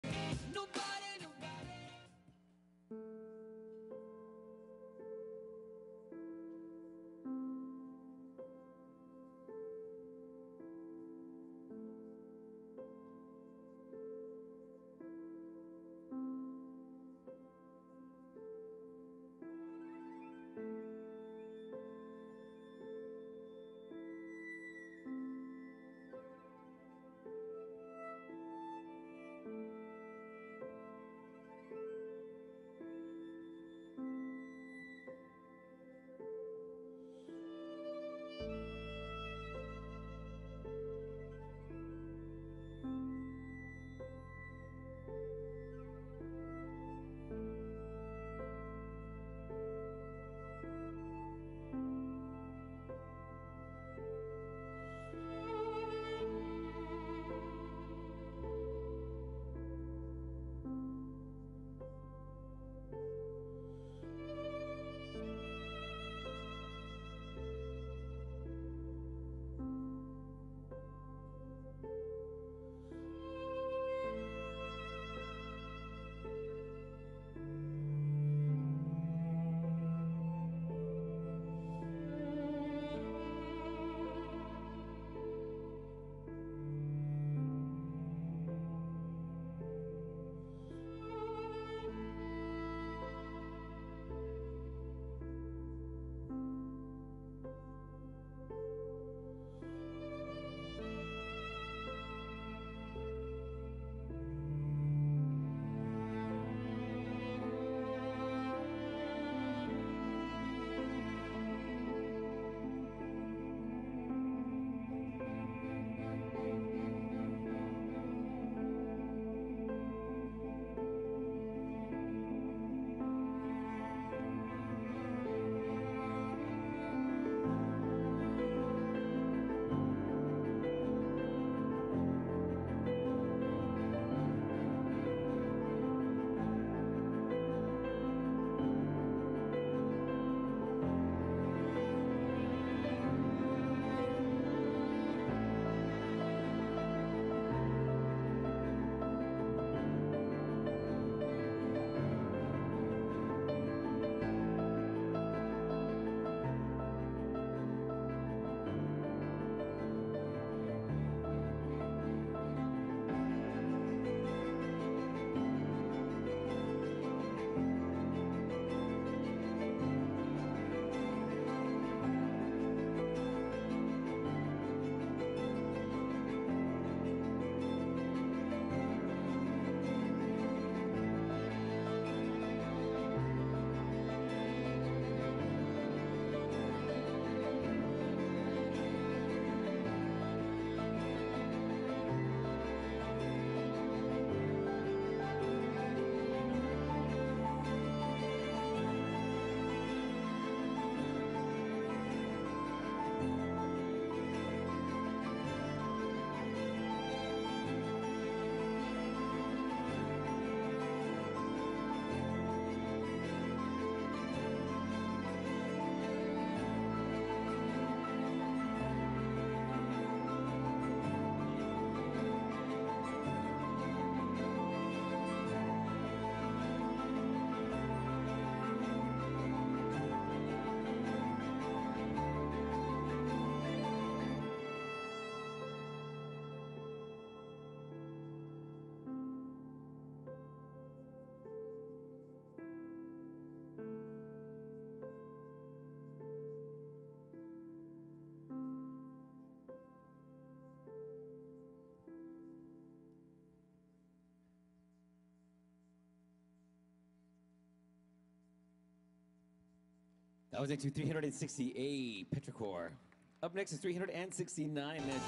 Petrichor - Prestige Academy of Dance
2nd Senior Group - You Rocked Jump!
Highest Scoring Senior Contemporary!
Senior Best of Jump!

Senior Contemporary Group
Prestige Academy of Dance
Jump Philadelphia 2020